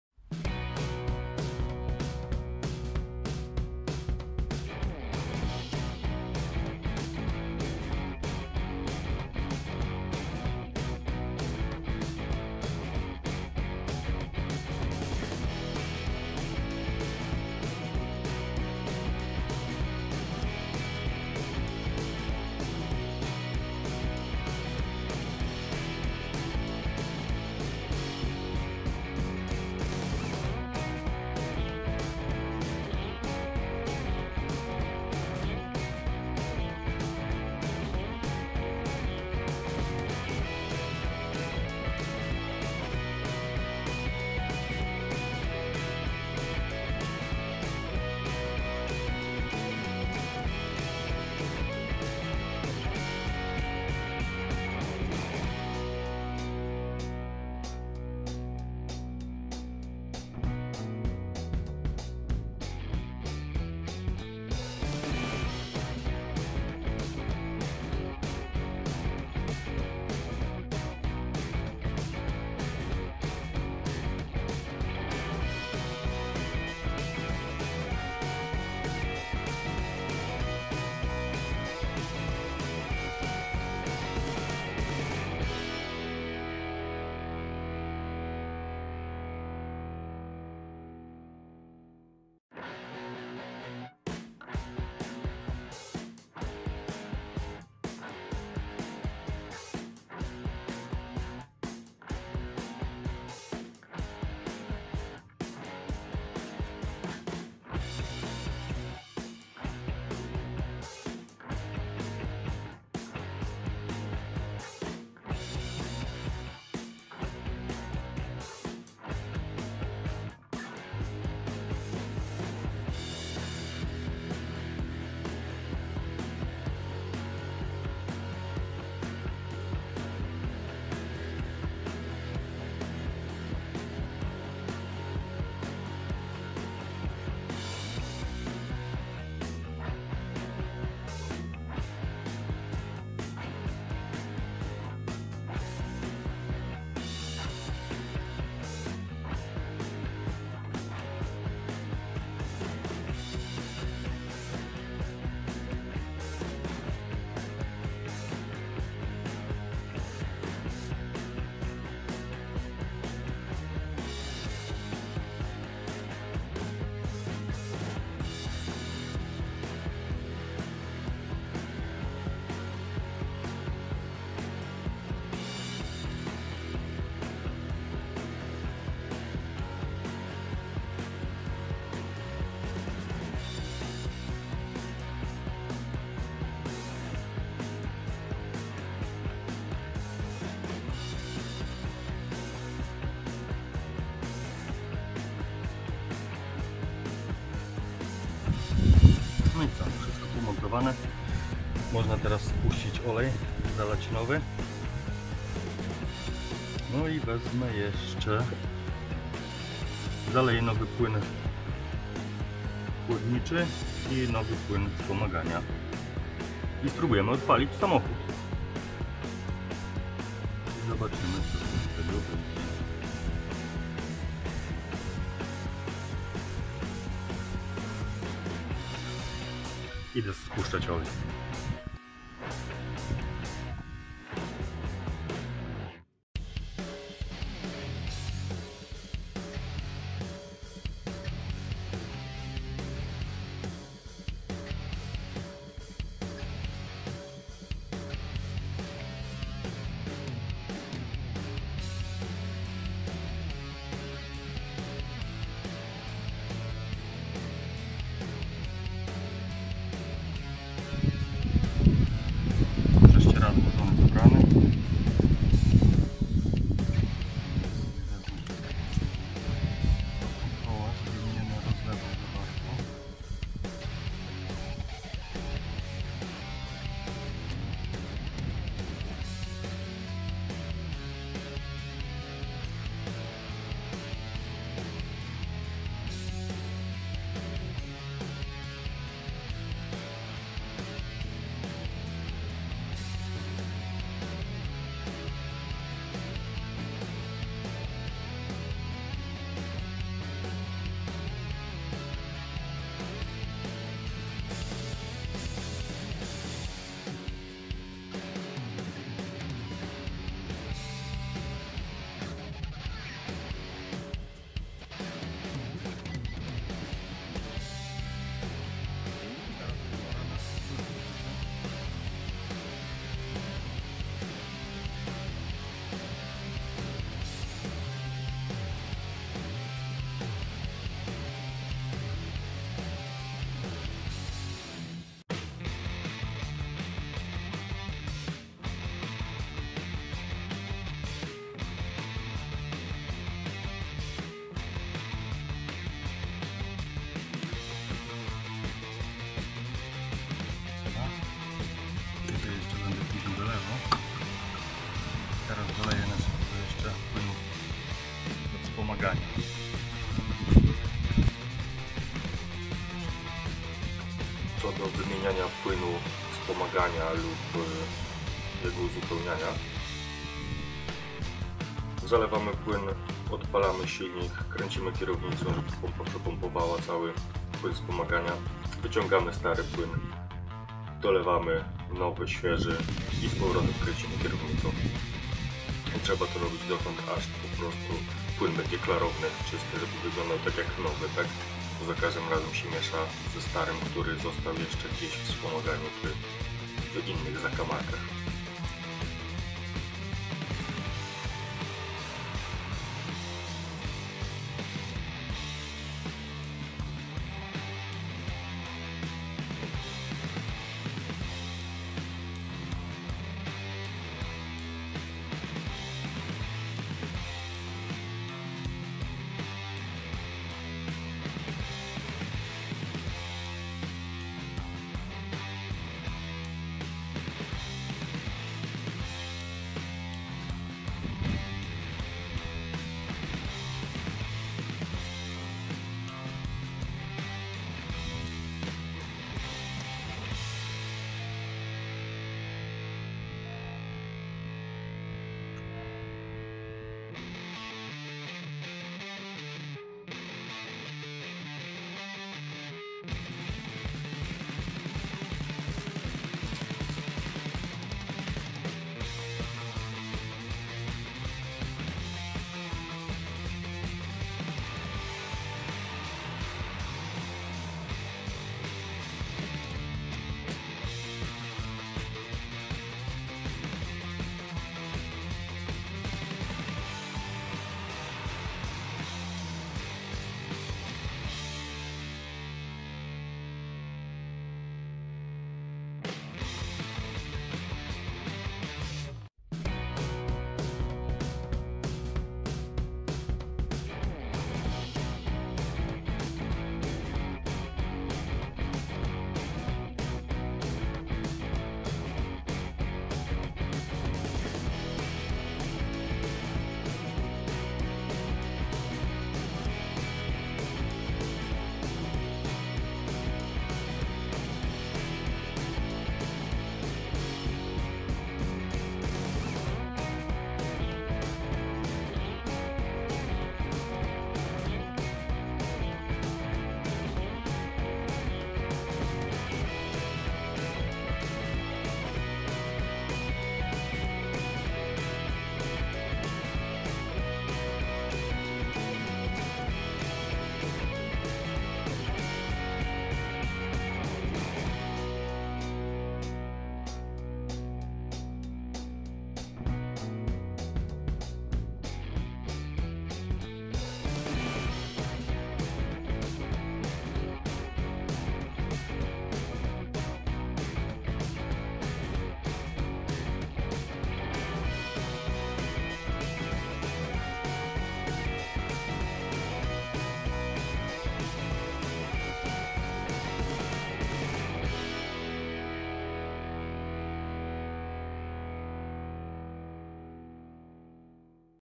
mercedes190# Montaż pompy wspomagania - kończenie prac
Nareszcie koniec , auto poskładane i można odpalać motor ,dzięki tej naprawie  sporo się nauczyłem o tym mercedesie , już doczekać się nie mogę realizacji dalszych planów :)